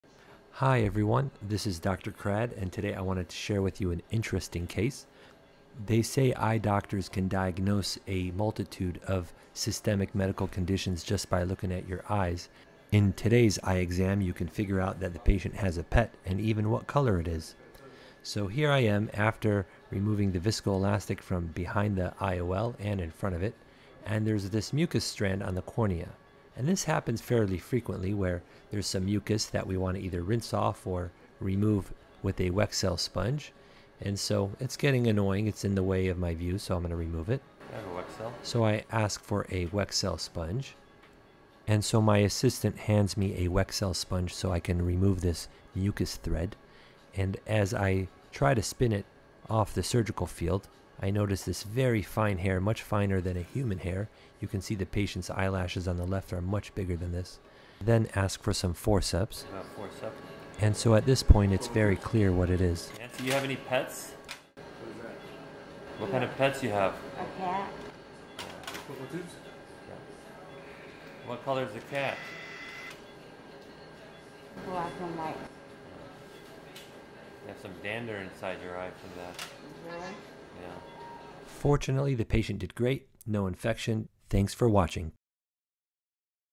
Cat Hair in the Eye
⚡ Dr. Omar Krad is a Board-Certified Ophthalmologist specializing in Cataract and Refractive Surgery.  His practice is in Orange County, California.

In this video, we discover a cat hair in the eye during cataract surgery!  Fortunately the betadine prep at the beginning of the case likely killed all/most of the germs in the eye, decreasing the risk of infection.  The patient did great and didn't get an infection.